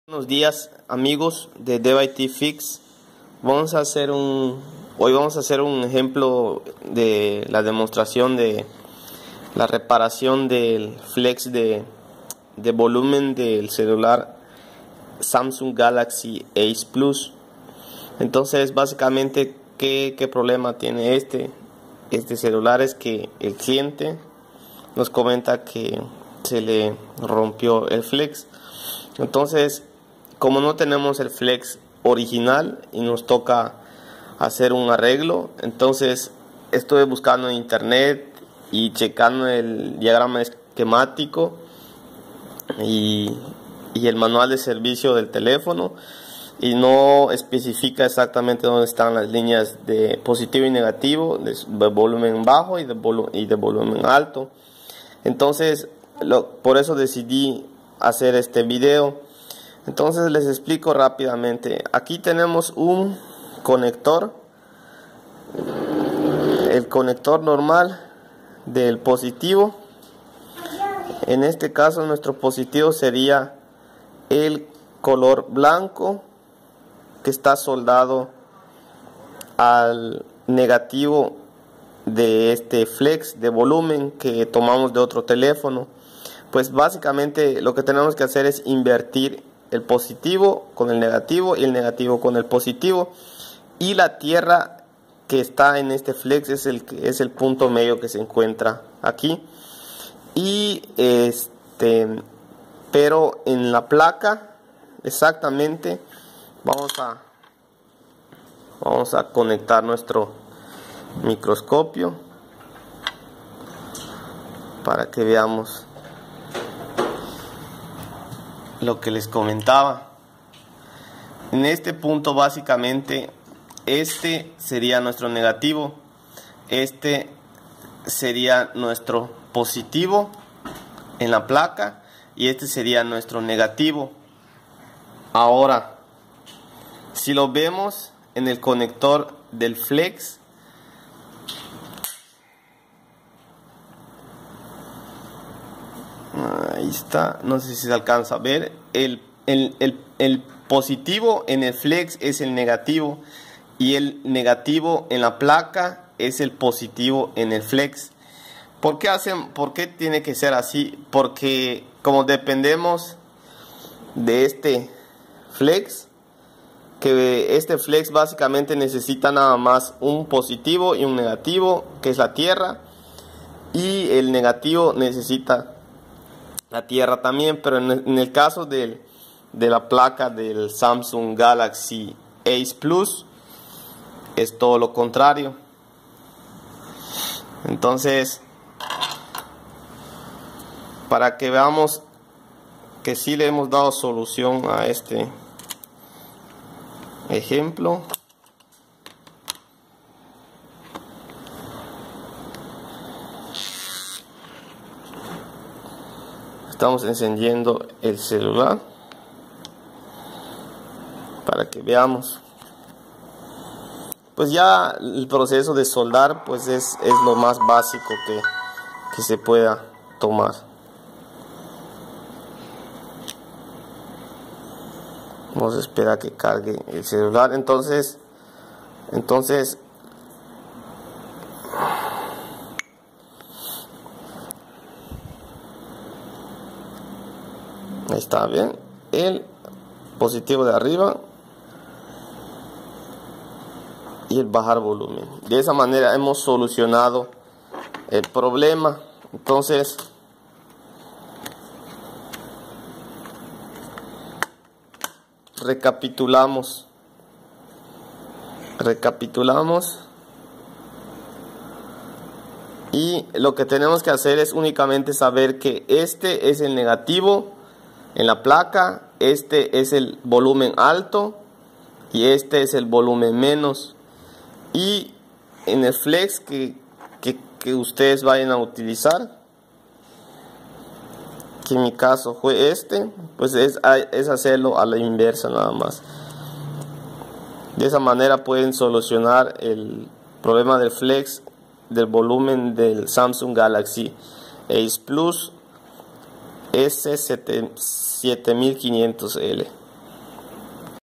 Reparar Flex volumen Samsung ACE plus
Reparación Flex volumen Samsung ACE plus, cuando hablo de negativo y positivo del Flex me refiero al volumen + y - lo no es la manera apropiada de referirse pero igual sirve ver el vídeo